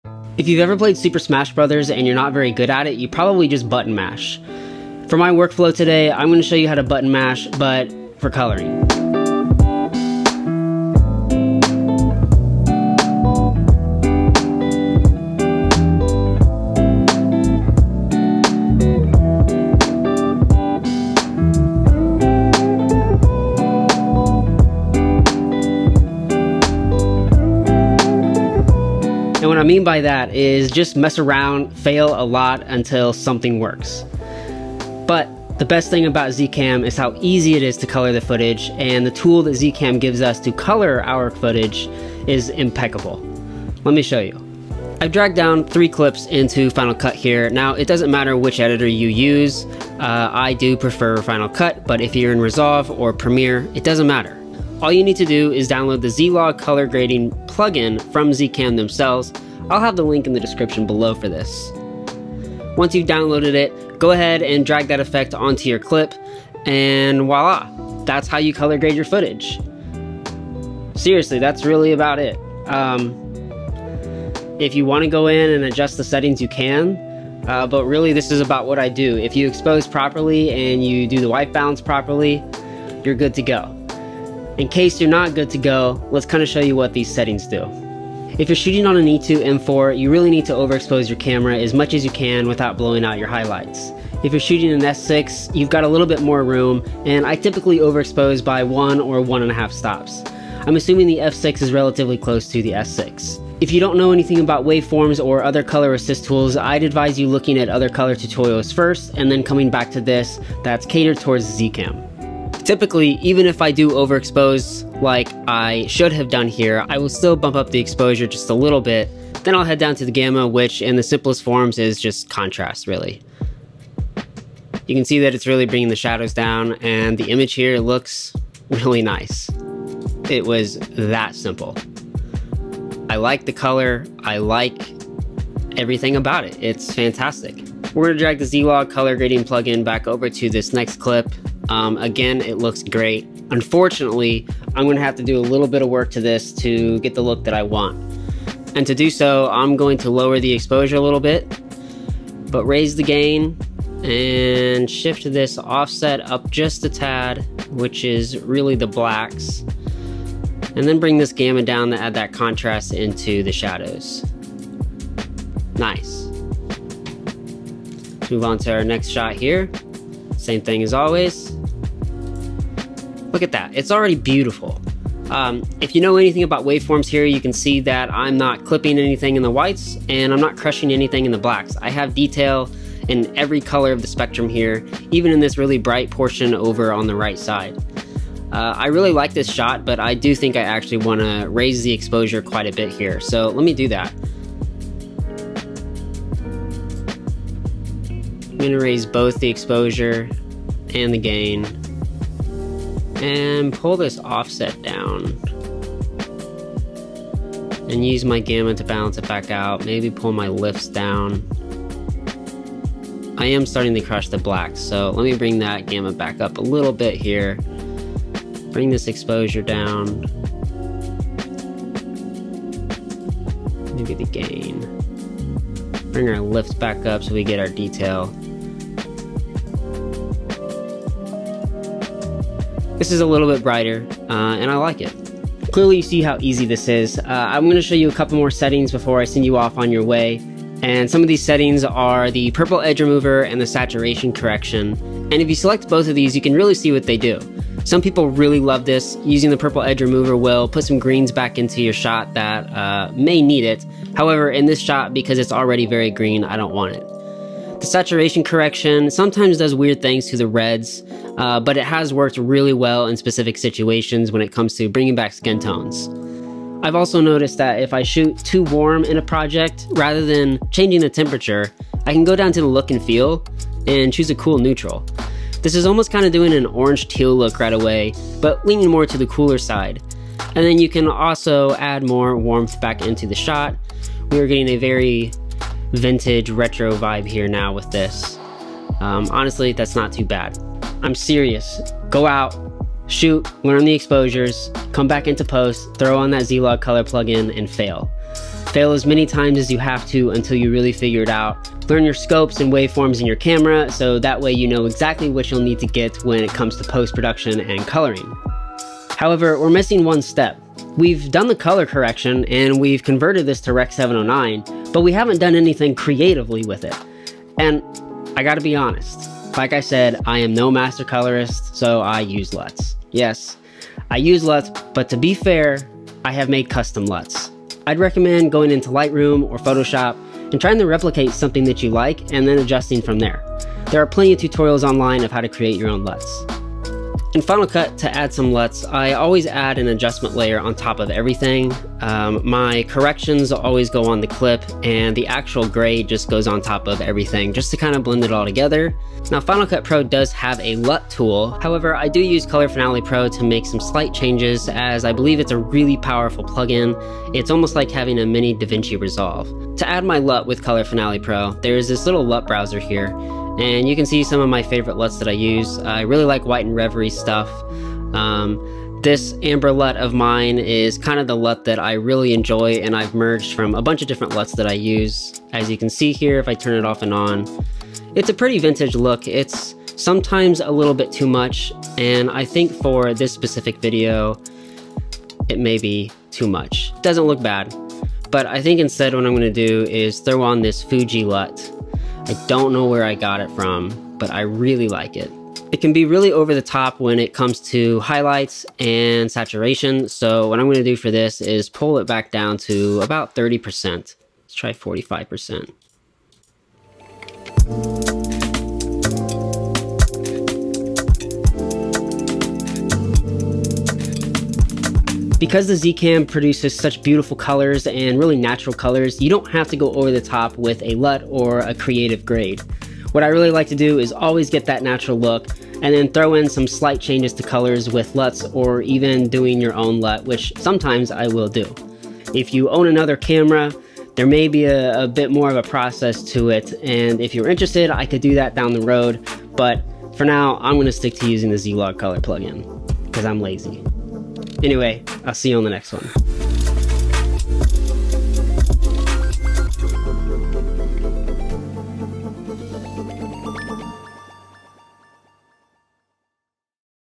Coloring Z-CAM Footage The Easy Way
Today I'm showing you how I easily and non-scientifically correct and grade my z cam footage. I choose to film in h.265 and proxy my footage for a more efficient workflow versus a prores option. I would highly advise watching an actual colorist if you want to dive deep into coloring.

Time Stamps:
Intro: 00:00
The Plugin: 00:44
Exposing: 01:27
Extra Settings: 03:55
Luts?: 05:08
Color Finale Pro: 05:49
Conclusion: 06:47